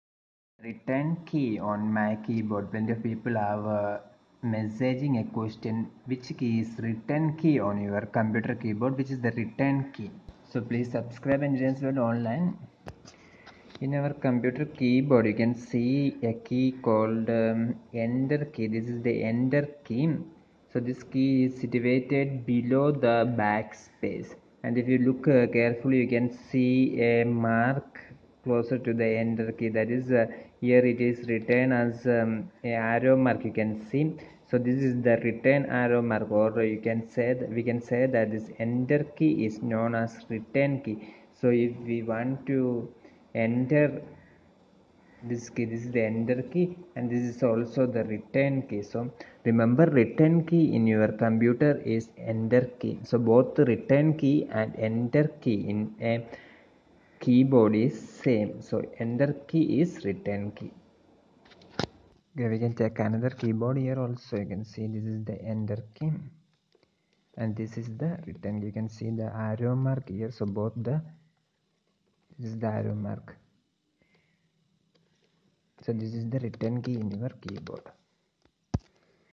Where is the return key on my pc keyboard?
Where is the return key on my keyboard?
Where is the return key on my pc?
Does Return key and enter key both are same?
Video answers yes for your confusion, if you are using windows including return key symbol.